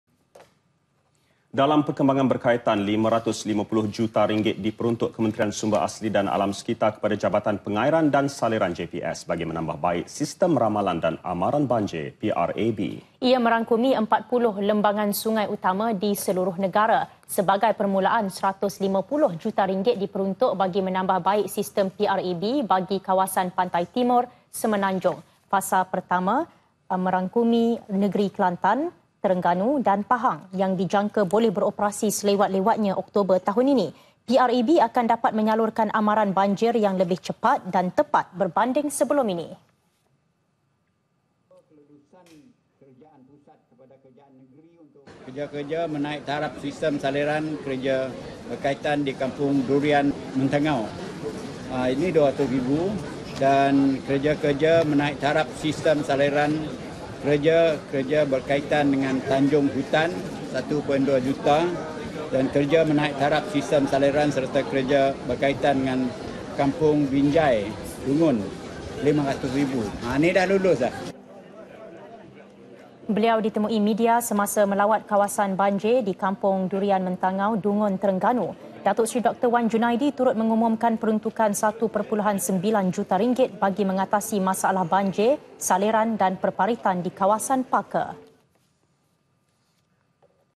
BANJIR PANTAI TIMUR- SISTEM RAMALAN DAN AMARAN BANJIR DI 40 SUNGAI DITAMBAH BAIK [8 JAN 2017]
550 juta ringgit diperuntuk Kementerian Sumber Asli dan Alam Sekitar kepada Jabatan Pengairan dan Saliran, JPS, bagi menambah baik sistem Ramalan dan Amaran Banjir - PRAB. PRAB merangkumi 40 lembangan sungai utama di seluruh negara.